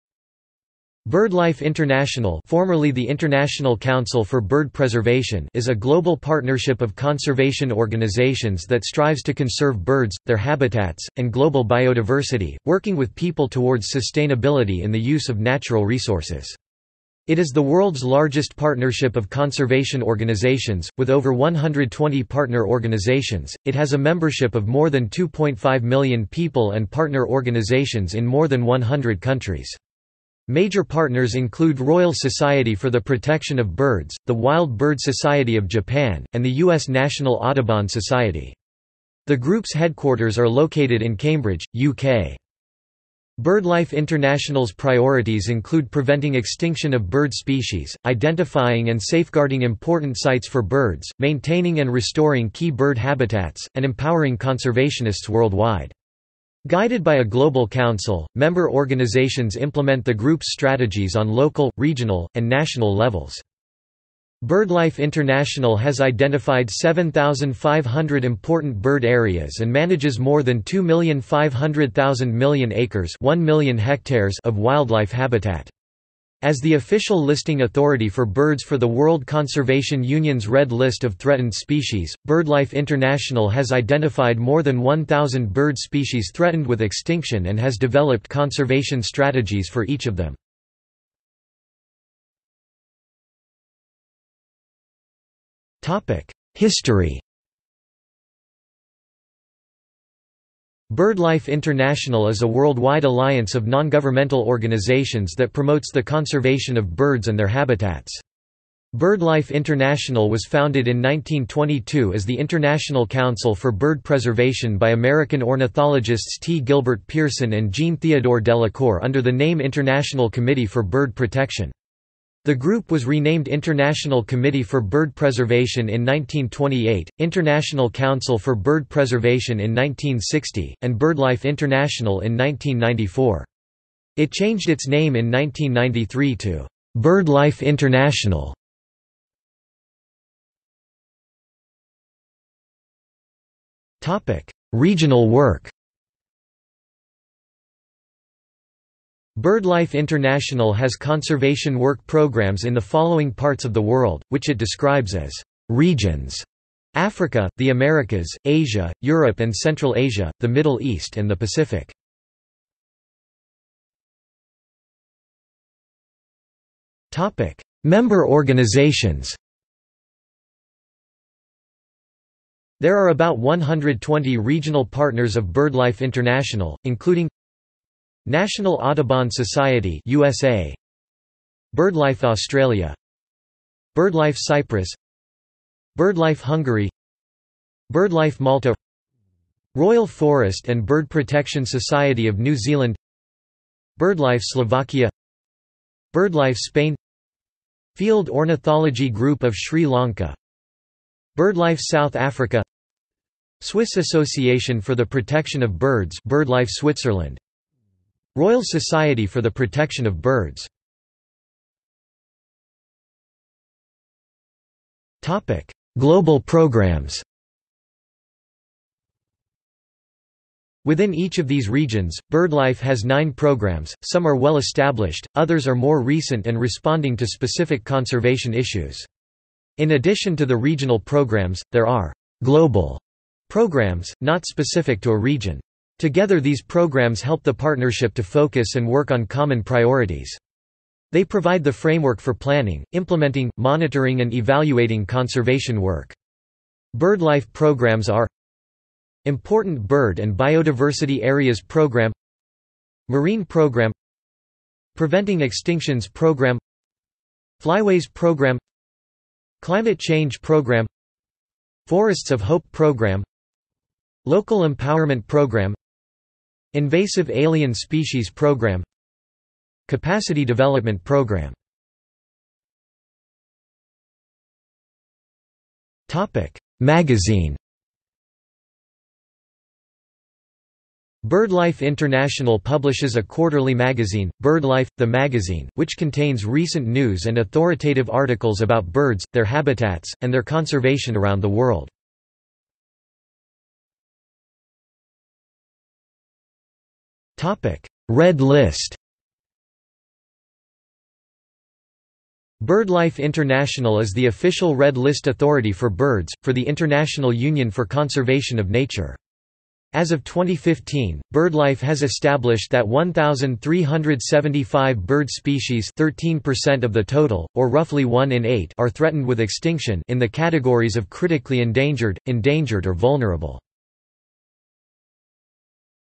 BirdLife International | Wikipedia audio article
This is an audio version of the Wikipedia Article:
BirdLife International

00:01:32 1 History
00:02:20 2 Regional work
00:02:43 2.1 Member organisations
00:03:31 3 Global programmes
00:04:32 4 Magazine
00:04:53 5 Red List

- Socrates

SUMMARY
BirdLife International (formerly the International Council for Bird Preservation)  is a global partnership of conservation organisations that strives to conserve birds, their habitats, and global biodiversity, working with people towards sustainability in the use of natural resources. It is the world's largest partnership of conservation organisations, with over 120 partner organisations.It has a membership of more than 2.5 million people and partner organizations in more than 100 countries. Major partners include Royal Society for the Protection of Birds, the Wild Bird Society of Japan, and the U.S. National Audubon Society. The group’s headquarters are located in Cambridge, UK.
BirdLife International’s priorities include preventing extinction of bird species, identifying and safeguarding important sites for birds, maintaining and restoring key bird habitats, and empowering conservationists worldwide. Guided by a global council, member organizations implement the group’s strategies on local, regional, and national levels.
BirdLife International has identified 7,500 important bird areas and manages more than 2,500,000 million acres (1,000,000 hectares) of wildlife habitat. As the official listing authority for birds for the World Conservation Union’s Red List of threatened species, BirdLife International has identified more than 1,000 bird species threatened with extinction and has developed conservation strategies for each of them.